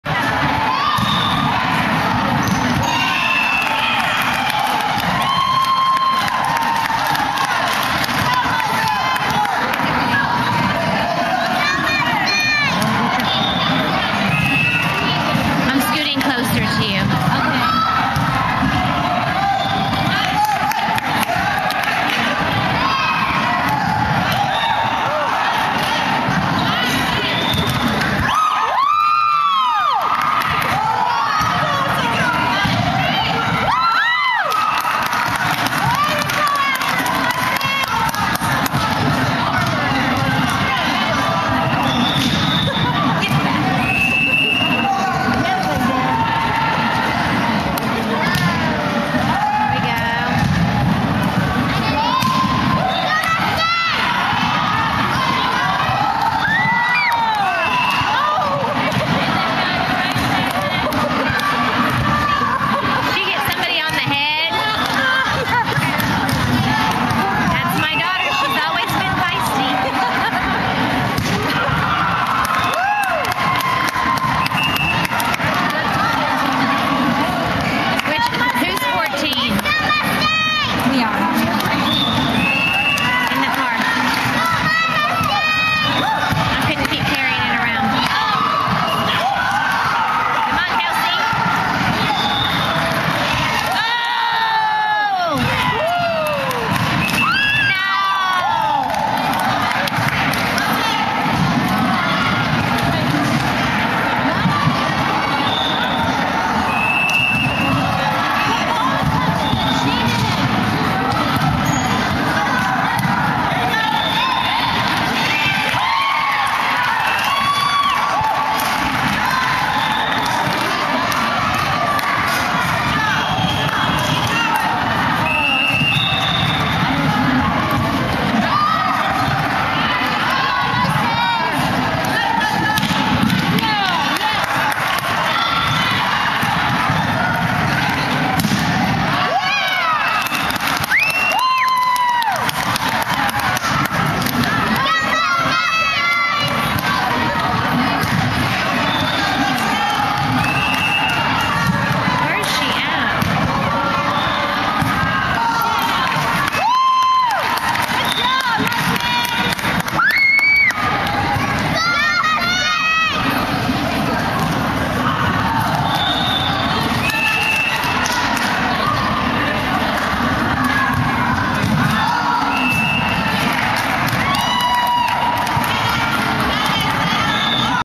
Mustangs volleyball bronze high team 08-09
My team is the blue. Oh and just letting you know I am number 22 setter, and yes, I did hit someone on the head. LOL Hope you enjoy!